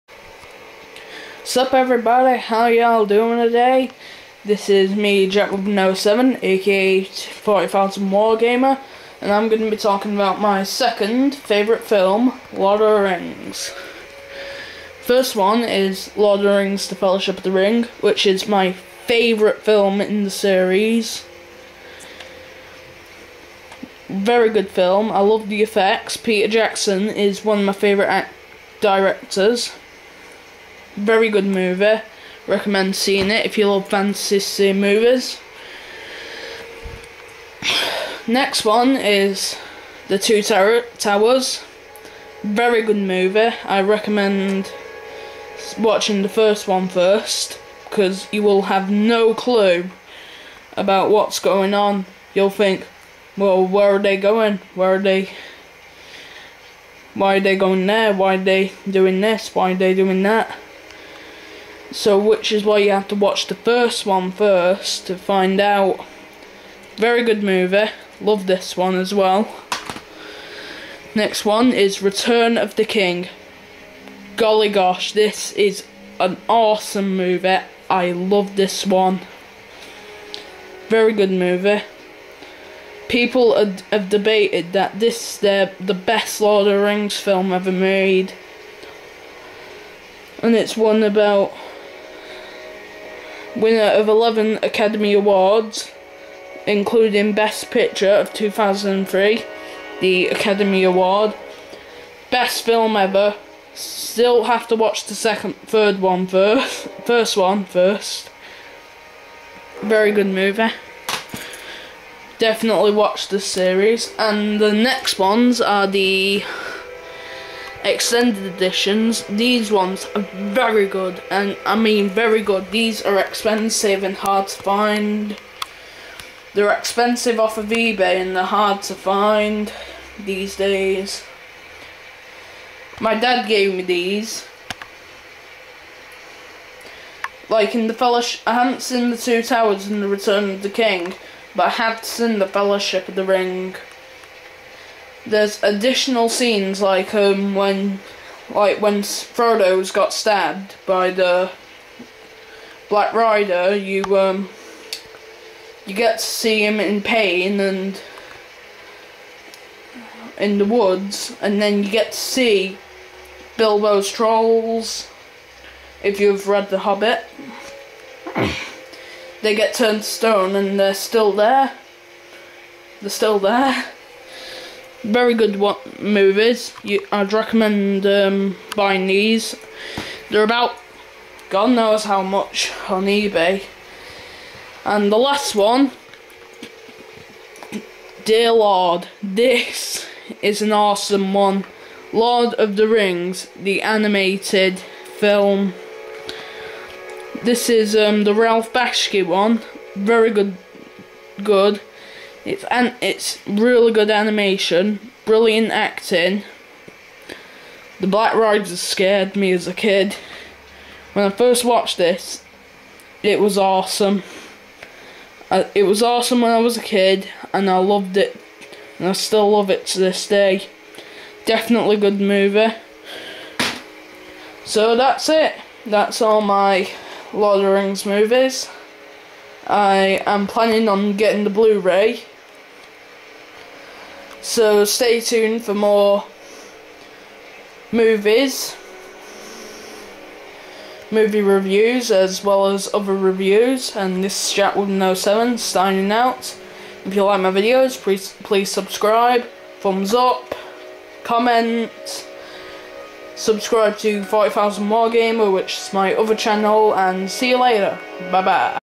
movie talk: lord of the rings
this is a video of me talking about lord of the rings my 2nd favourite movie